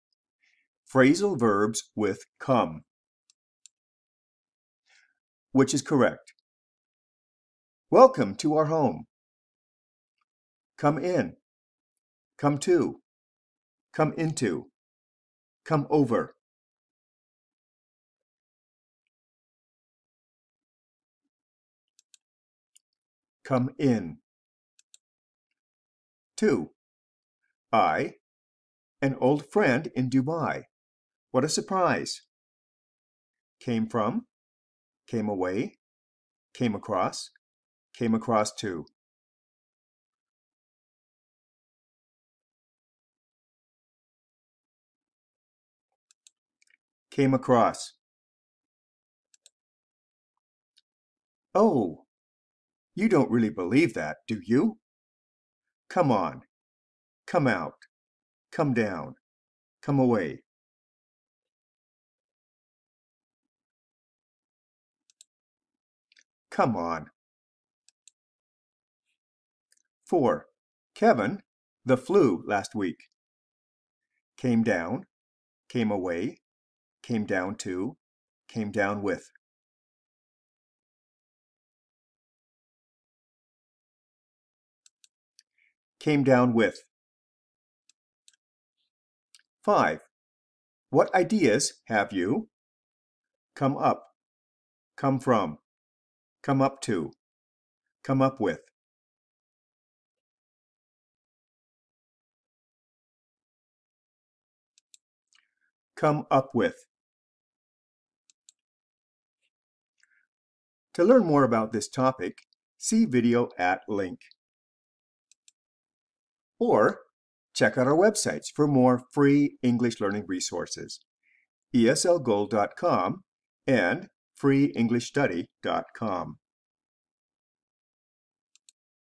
Phrasal Verbs in English: Come
The video deals with phrasal verbs with "come."